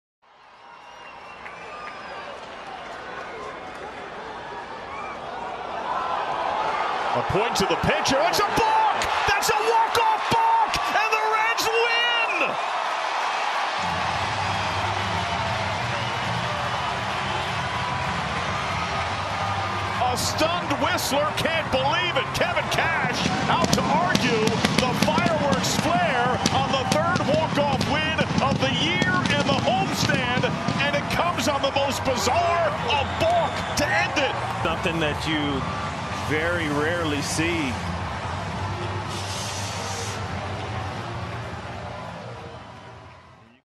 THE BALKOFF! Cincinnati Beats Tampa Bay on a Walk-off Balk (Reds/Rays | 7/8/22)
Umpire calls balk to gift Cincinnati walk-off win.